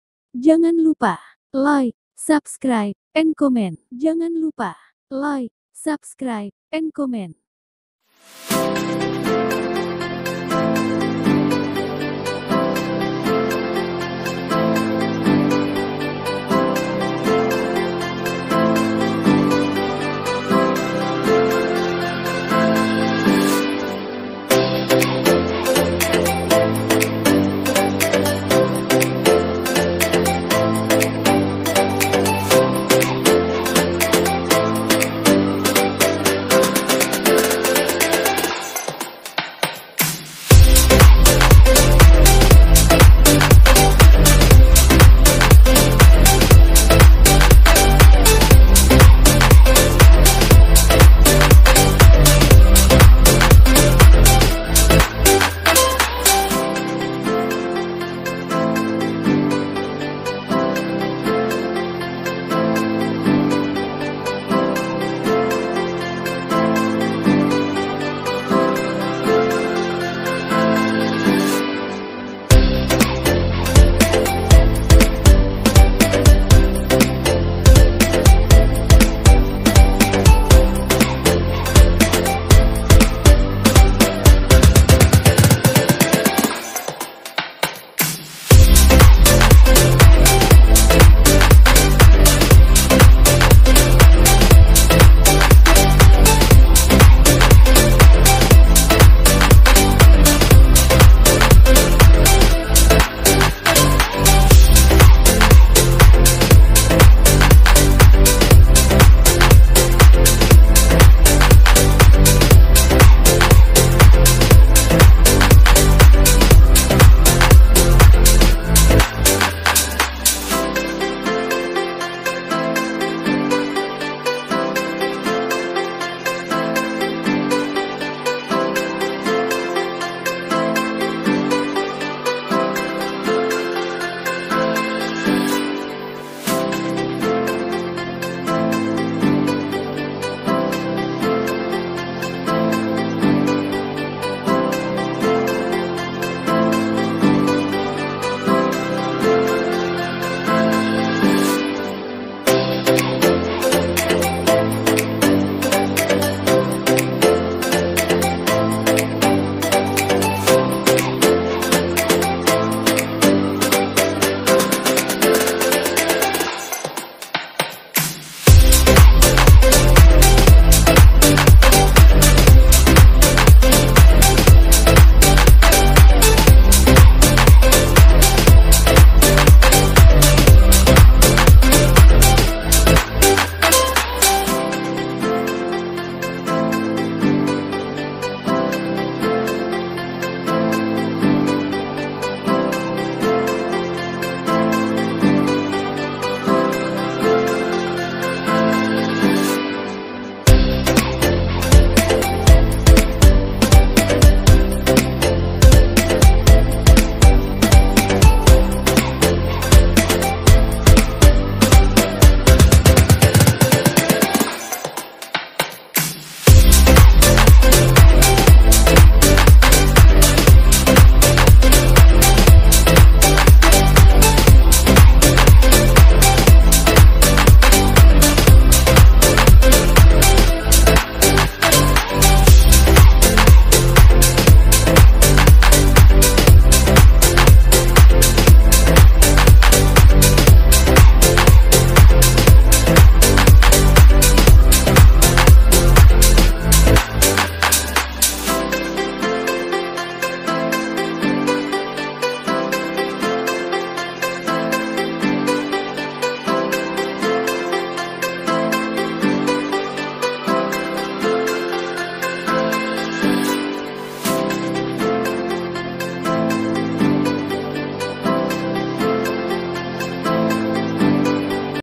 Dalam waktu 4 menit we can master 9 KINDS OF COOKING TOfu that can be sold and for snacking
tahu
we can master 9 KINDS OF COOKING TOfu that can be sold and for snacking